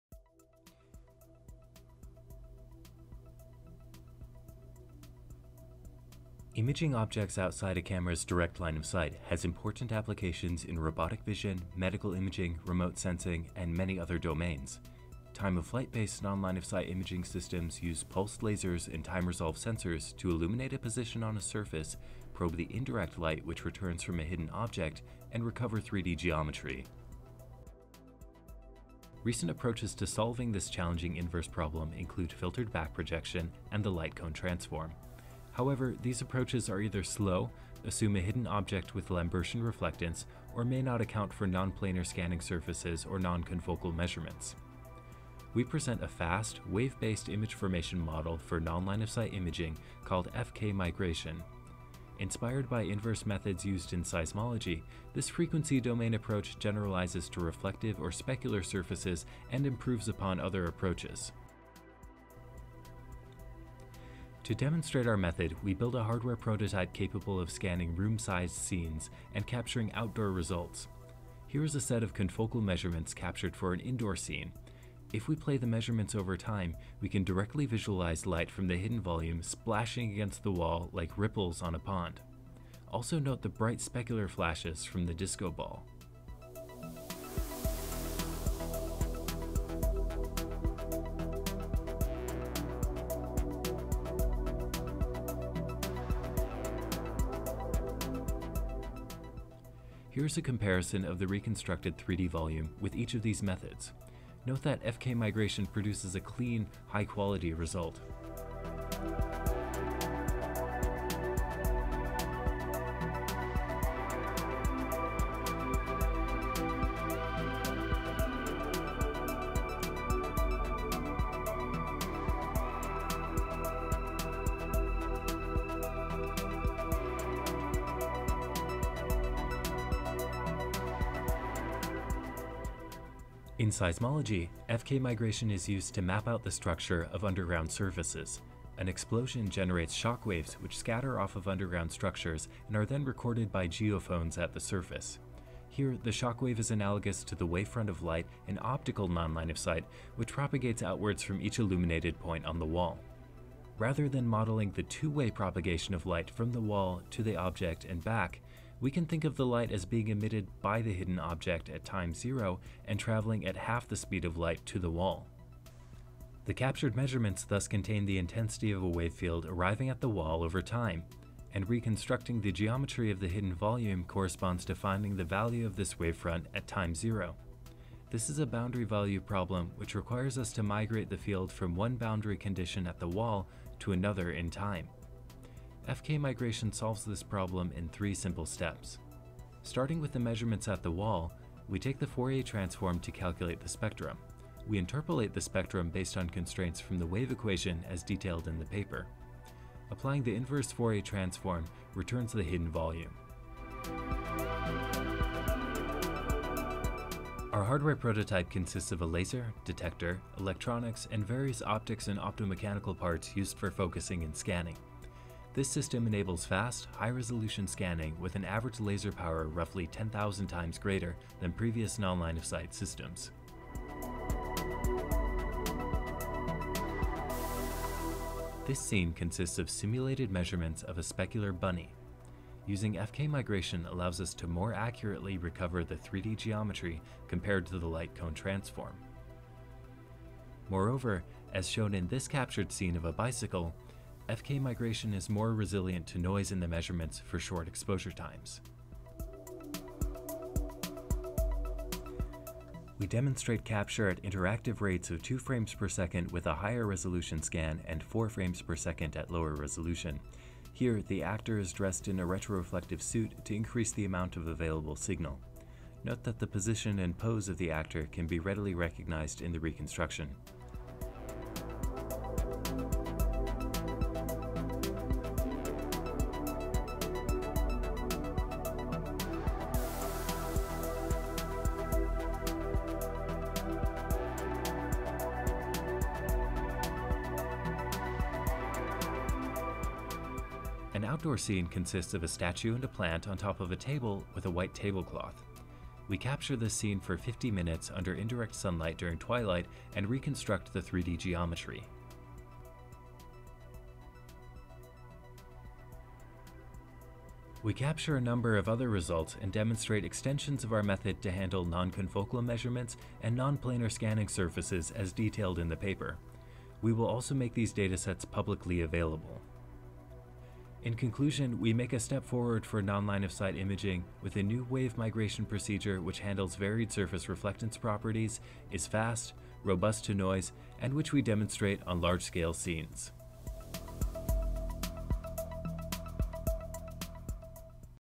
Wave-Based Non-Line-of-Sight Imaging using Fast f–k Migration | SIGGRAPH 2019
We introduce a wave-based image formation model for the problem of non-line-of-sight (NLOS) imaging. Inspired by inverse methods used in seismology, we adapt a frequency-domain method, f-k migration, for solving the inverse NLOS problem.